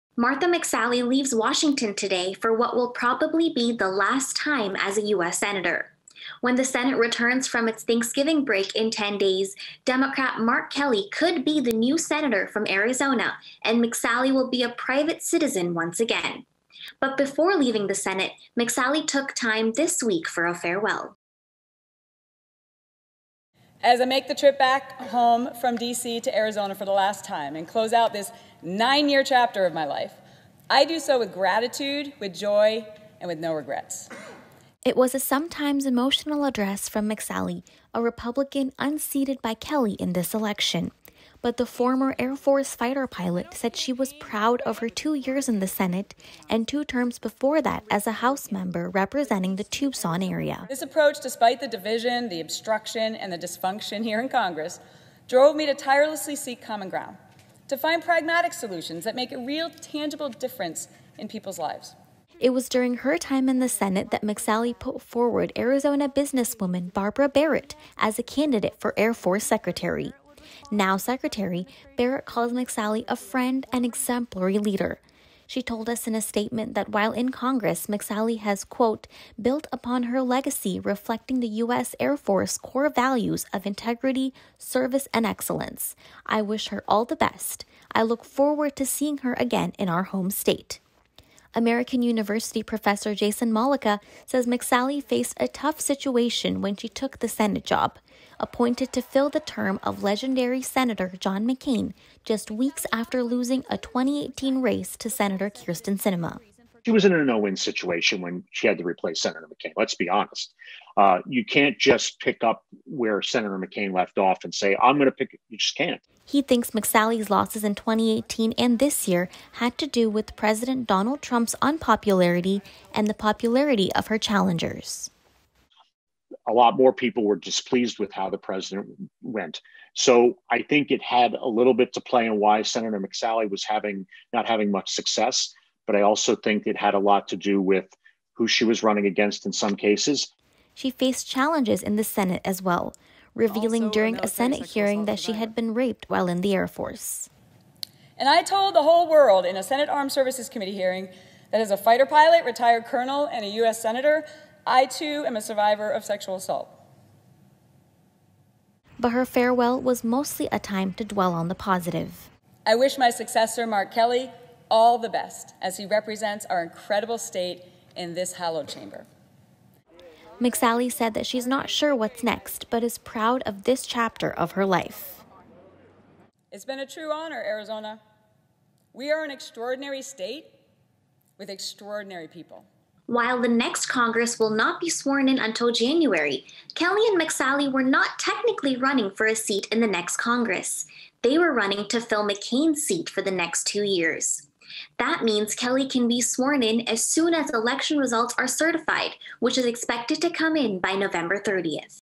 McSally Farewell | Cronkite News
After an emotional farewell speech on the floor of the Senate Wednesday, Sen. Martha McSally, R-Arizona, gets on a plane from DC for the last time today. Senate is on recess next week and she could be replaced by Mark Kelly as soon as Nov. 30. (Video by Mythili Gubbi/Cronkite News)